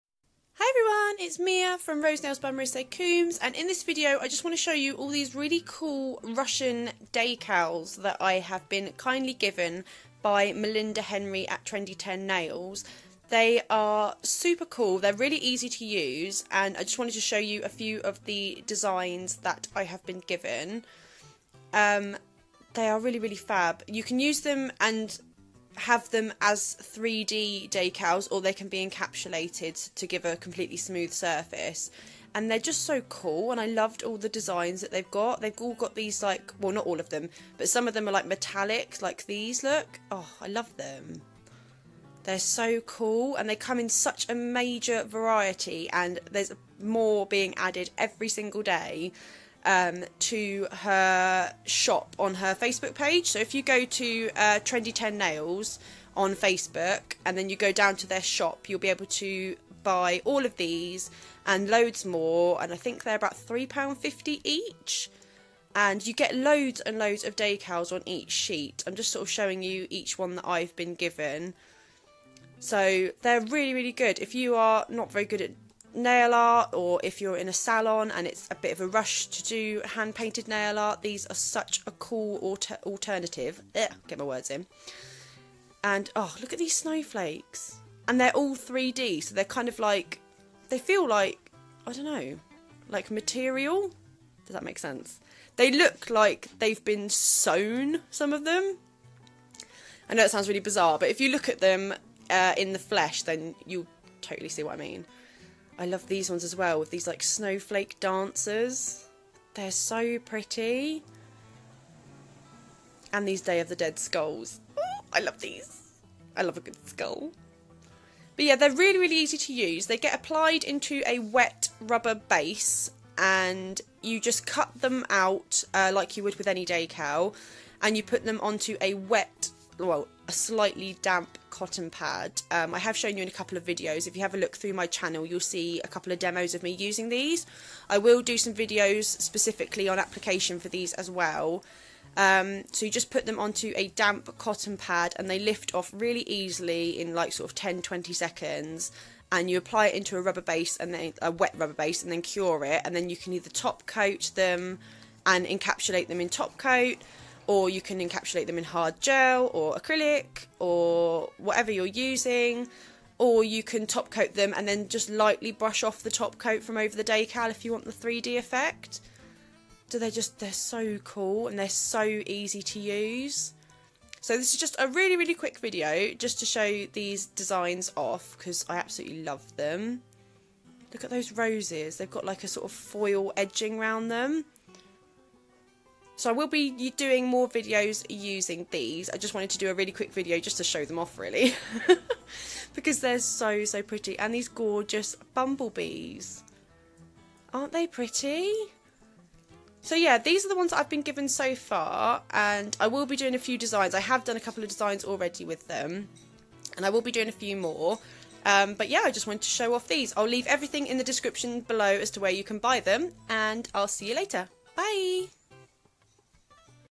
3d water decal sliders!!!!!
This is just a selection of the 3d decals available from Trendy10 :)

Click the link below to view and purchase :)

Please consider following me on Facebook!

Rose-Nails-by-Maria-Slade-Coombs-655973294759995